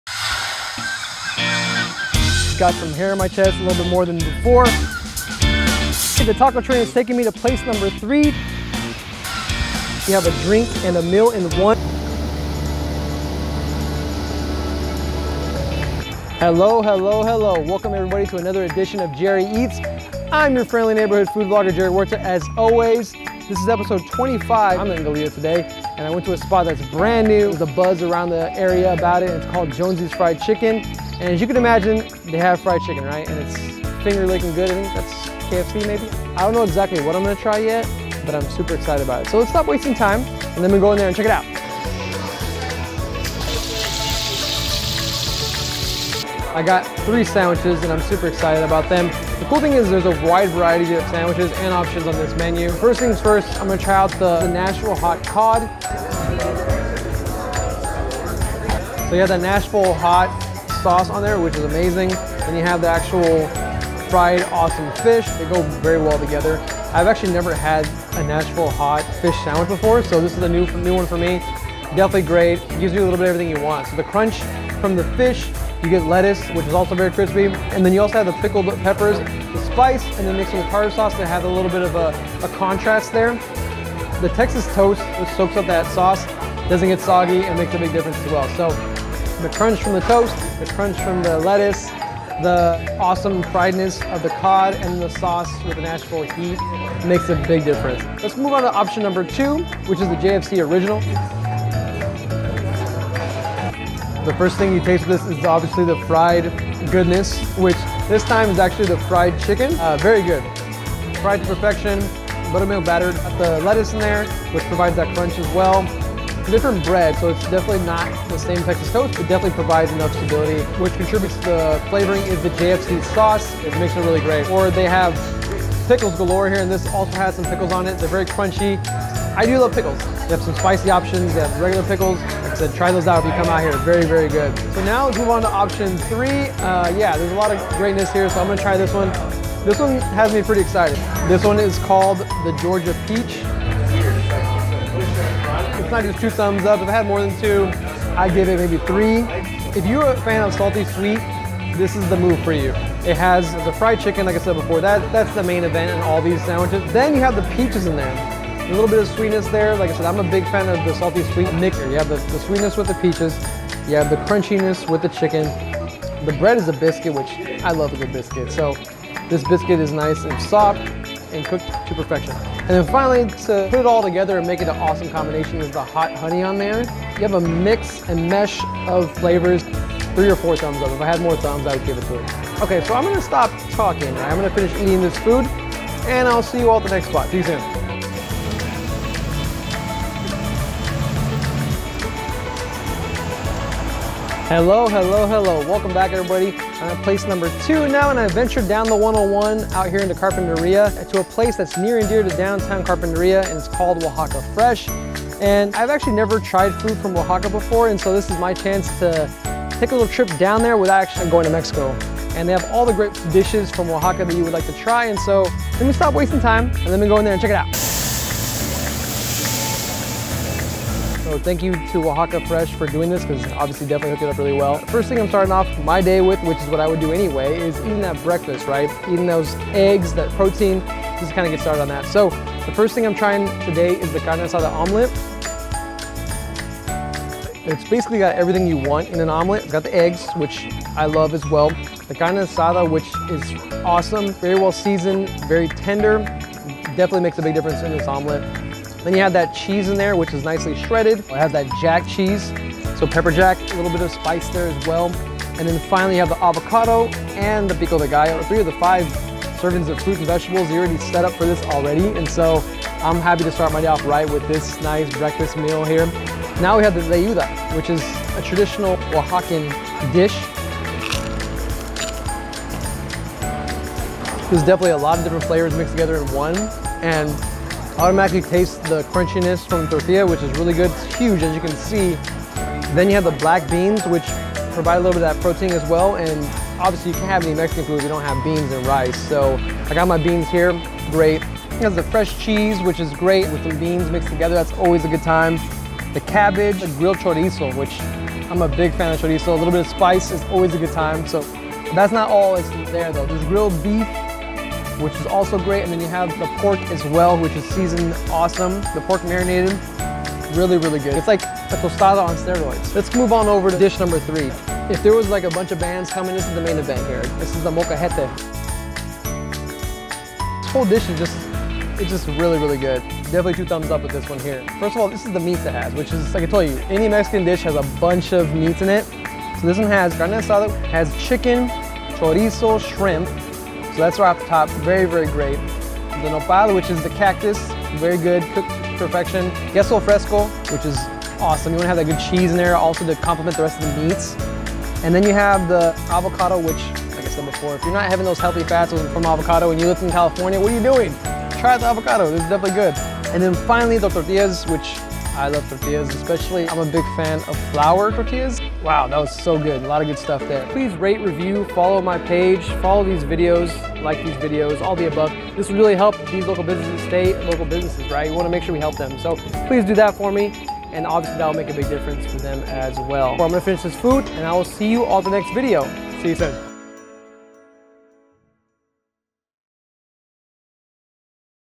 JerryEats: Episode 25
The food journey continues for Jerry to try all of the food in the 805. He visited Jonesy's Fried Chicken in Goleta and Oaxaca Fresh in Carpinteria.